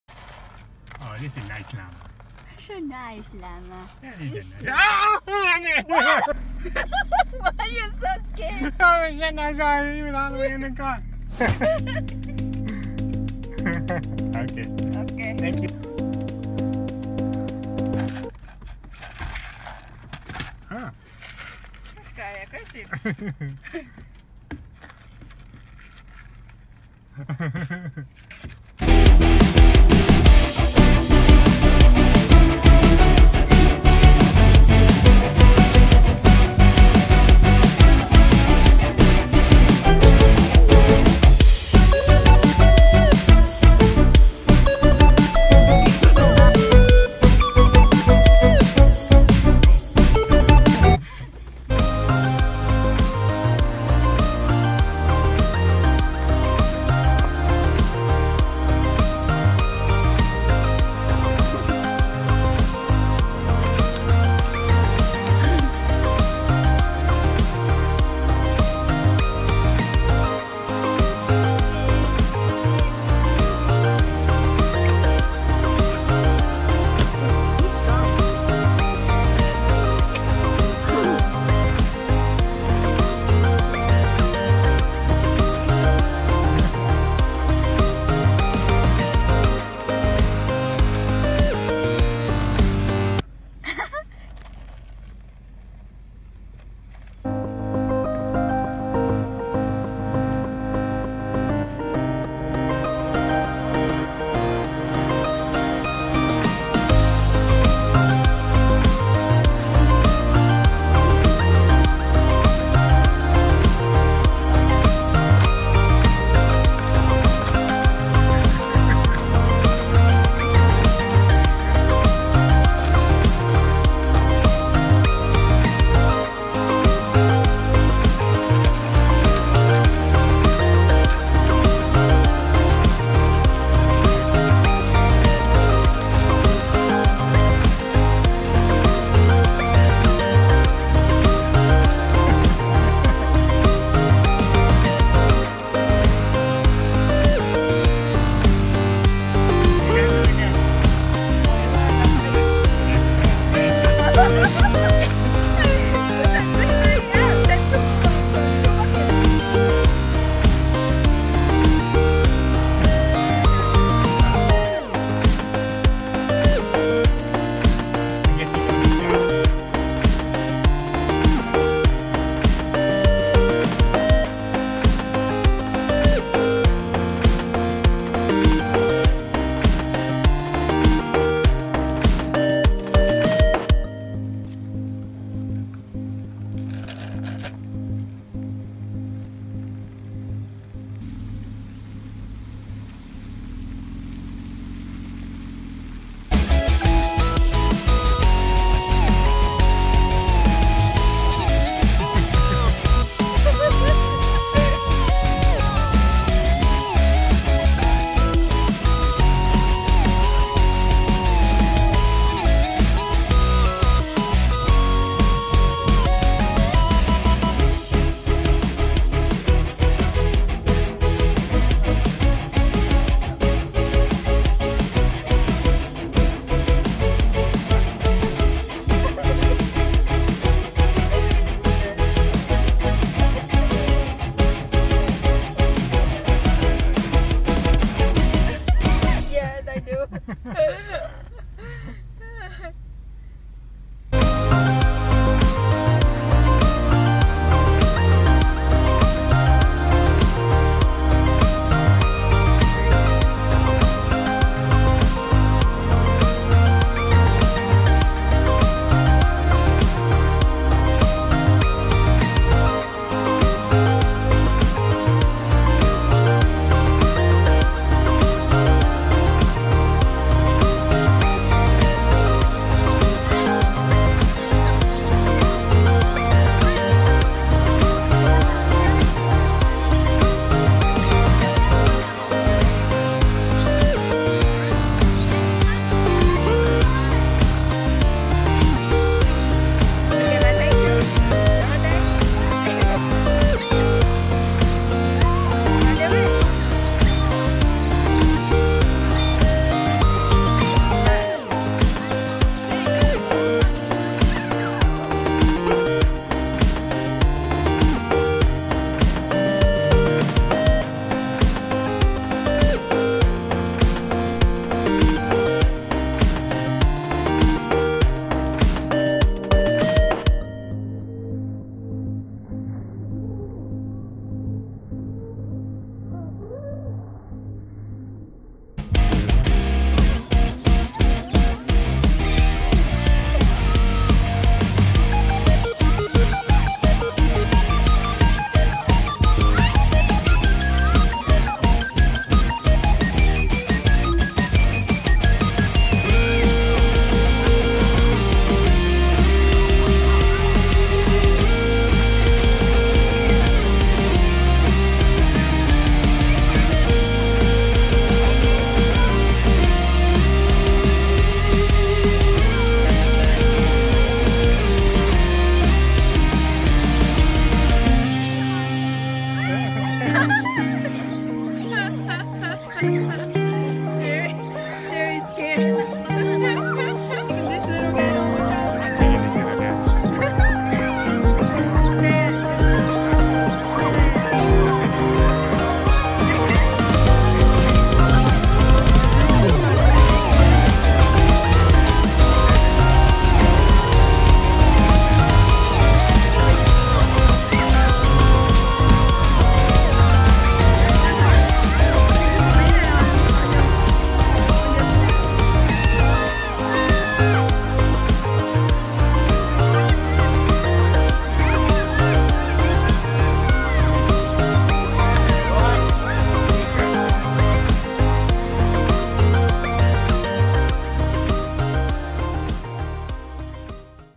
3 Tips When Visiting Virginia Safari Park to Get the Most Fun!
This video is about Virginia Safari Park. We drove through twice. We went to the petting zoo, the bird feeding was so much fun. We could easily spend the whole day here, but we had to leave to go to other attraction nearby.